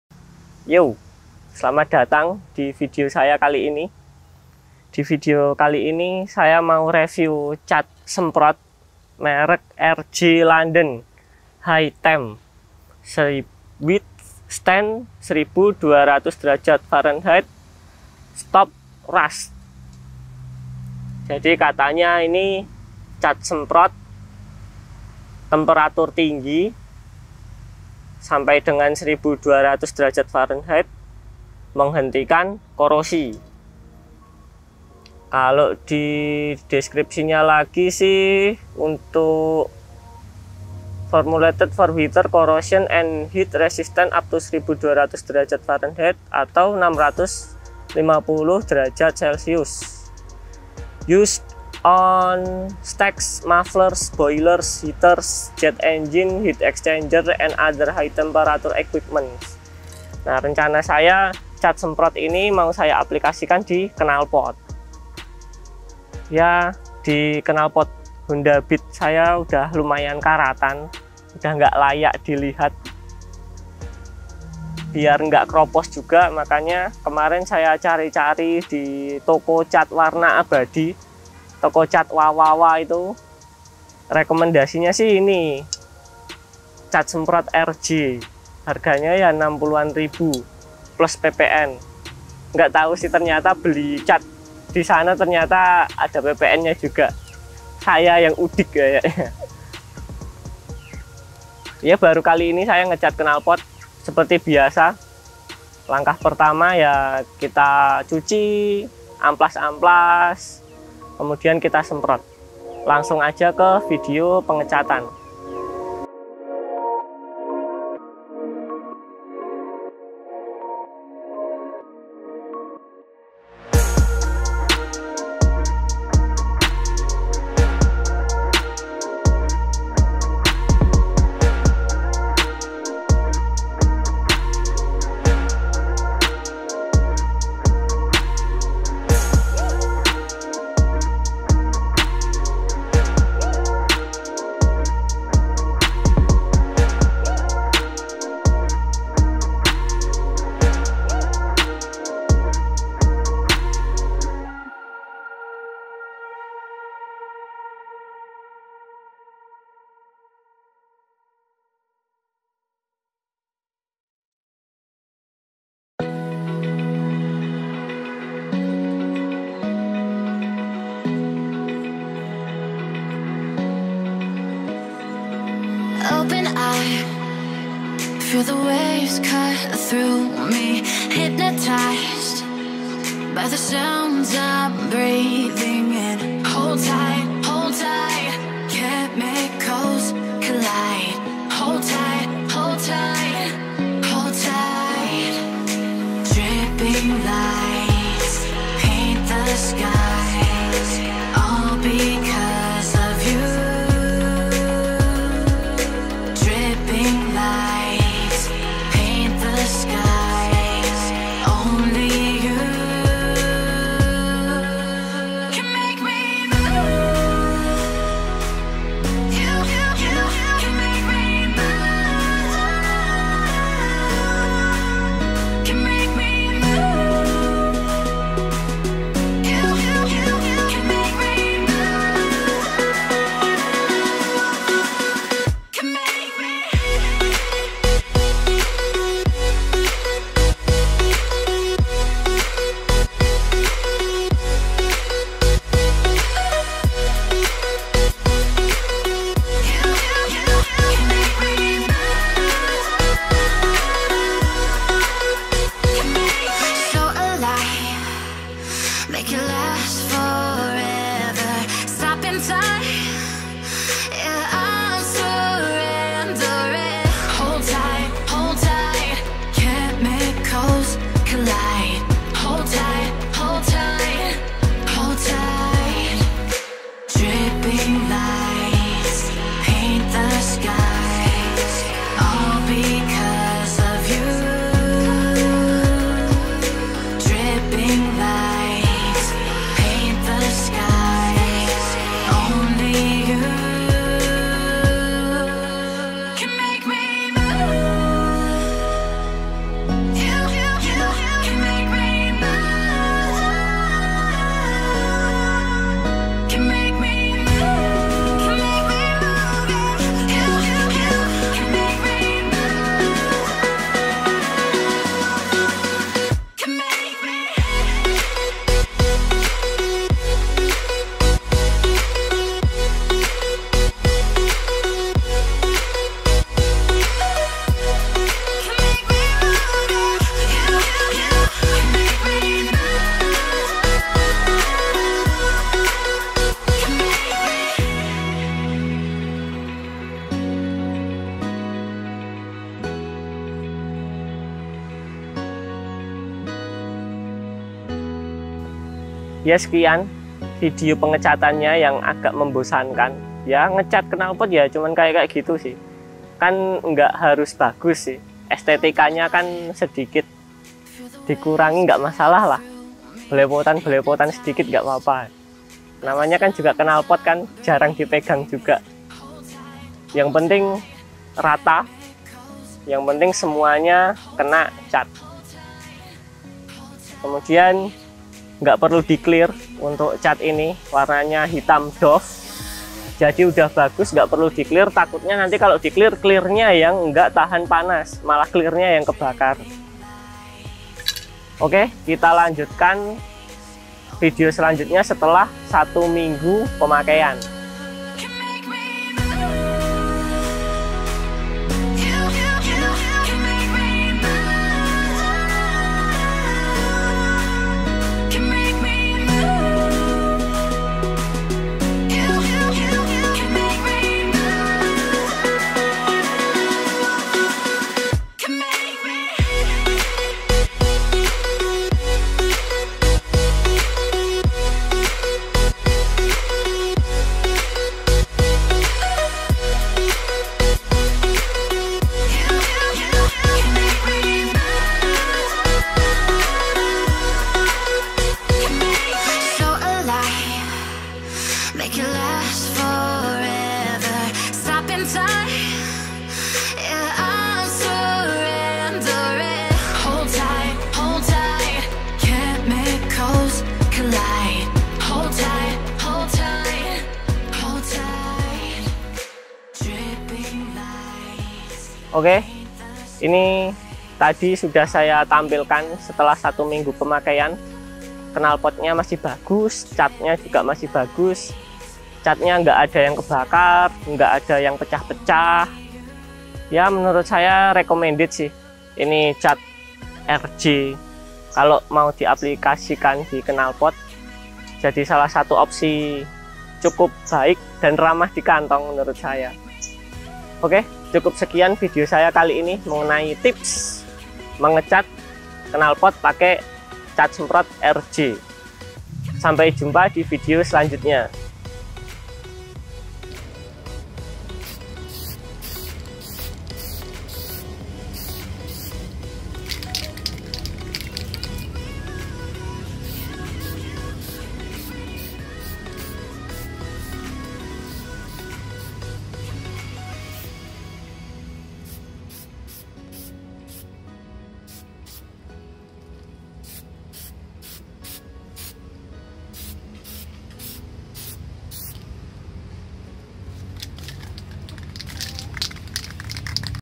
Cat Knalpot Murah & Mudah Pakai Cat Semprot RJ Temperatur Tinggi
Ngecat knalpot sendiri dirumah gampang & murah pakai cat semprot merek RJ, bisa di beli di toko warna cat abadi WA WA WA. ga perlu pakai paint remover, cukup cuci bersih & amplas saja secukup nya
lalu semprot tipis tipis, tunggu sampai kering, kemudian lapisi lagi, terus begitu sampai ketebalan yang kamu inginkan.
1 kaleng cat semprot RJ ini bisa di pakai untuk ngecat 2 knalpot
murah harga cuman sekitar 60rb, atau bahkan ga nyampe 60rb tergantung kebijakan toko nya.
Cat semprot RJ temperatur tinggi ini juga gampang kering, jadi ga usah di jemur terik matahari pun bisa kering. cuman tips ketika baru dipasang di knalpot jangan dipakai jarak jauh langsung seketika, di panasin dulu, biar kandungan tinner pada cat menguap sempurna, setelah itu perfect deh kondisi knalpot nya.